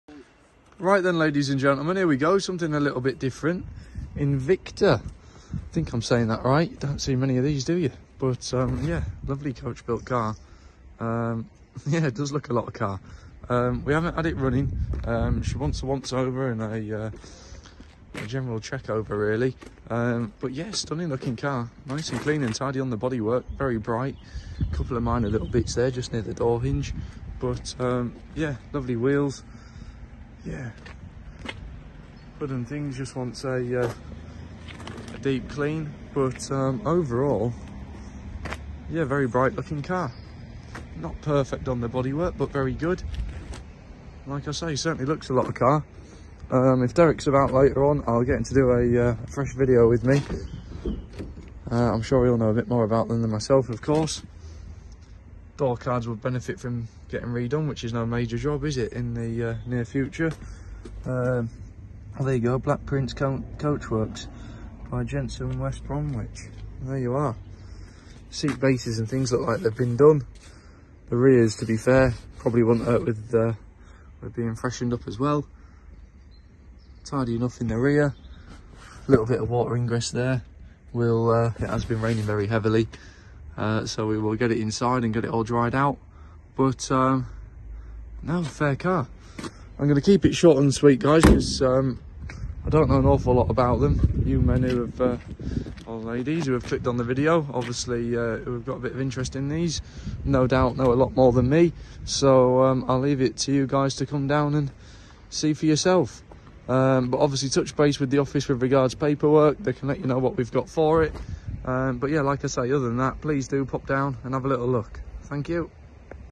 1950 INVICTA BLACK PRINCE | MATHEWSONS CLASSIC CARS | AUCTION: 1, 2 & 3 MAY 2024
1950 INVICTA BLACK PRINCE | MATHEWSONS CLASSIC CARS | AUCTION: 1, 2 & 3 MAY 2024

Join Derek, Dave, Paul and everyone at Mathewsons Classic Cars for our live online classic car, motorcycle and memorabilia auction on May 1, 2 & 3 2024.